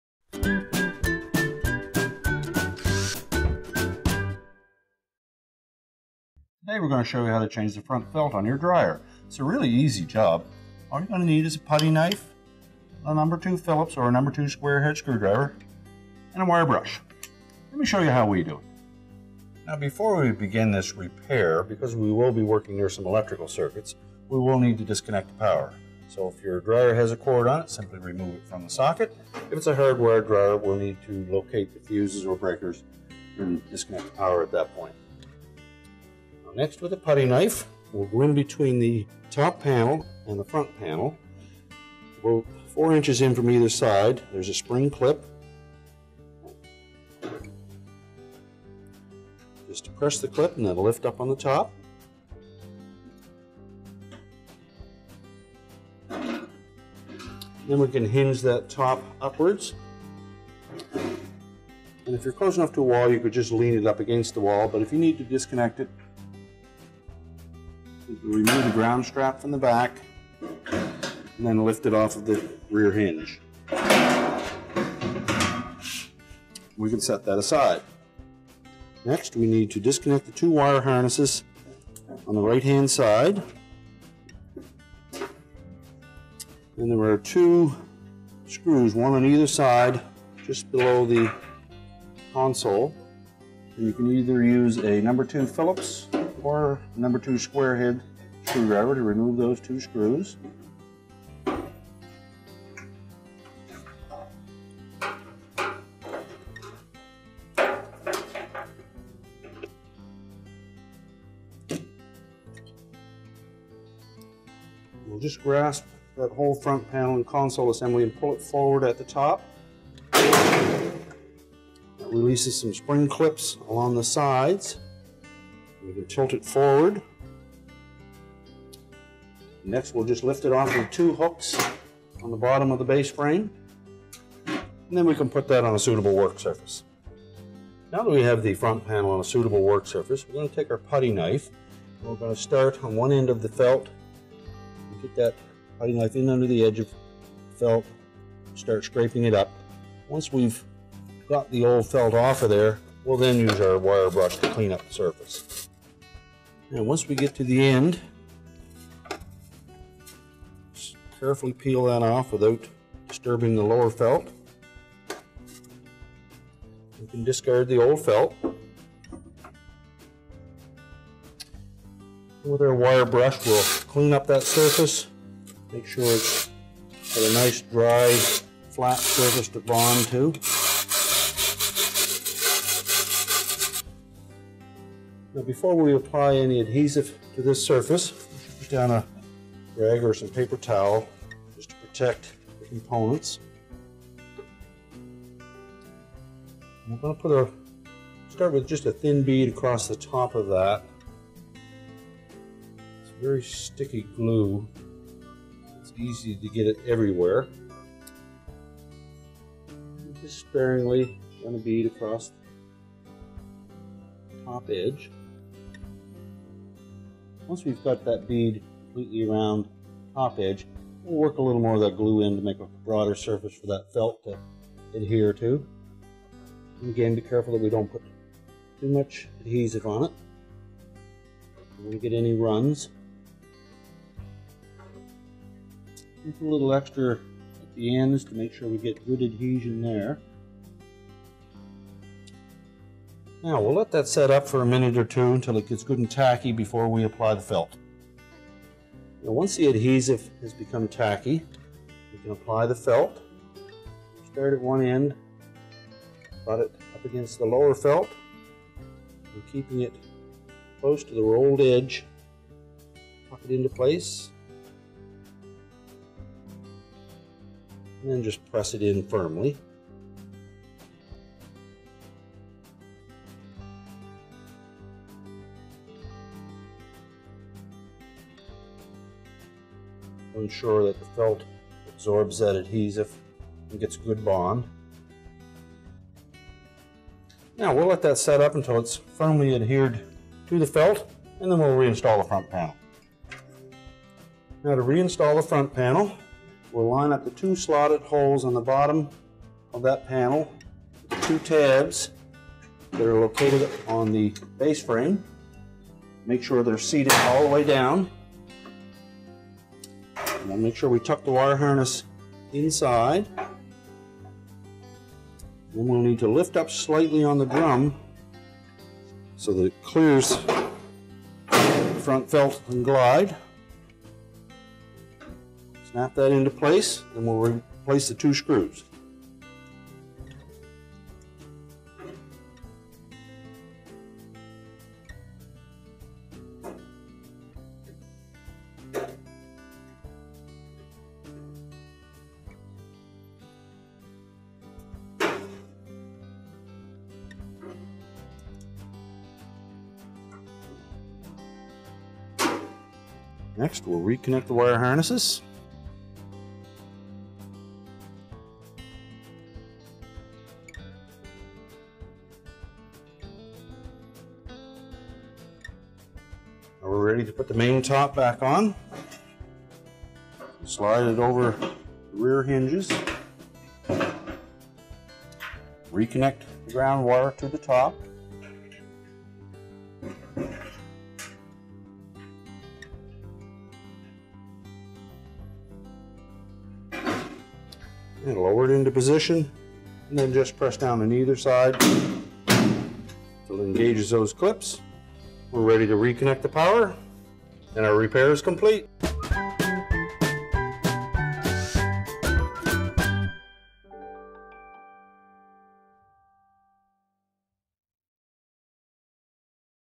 Frigidaire Dryer Repair – How to replace the Front Felt
Need help replacing the Front Felt (Part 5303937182) in your Frigidaire Dryer? Watch this how to video with simple, step-by-step instructions for a successful DIY repair.

Why you might need to replace the Front Felt:
1.    Noisy
2.    Will not tumble
3.    Marks left on clothes

What tools you need for this repair:
• Putty knife
• #2 Philips screw driver
• Wire brush

The information in this video is specific to Frigidaire, Kenmore, Westinghouse, Gibson, Crosley, Uni and Electrolux. To find a Front Felt specific to your model,

Not what you’re looking for?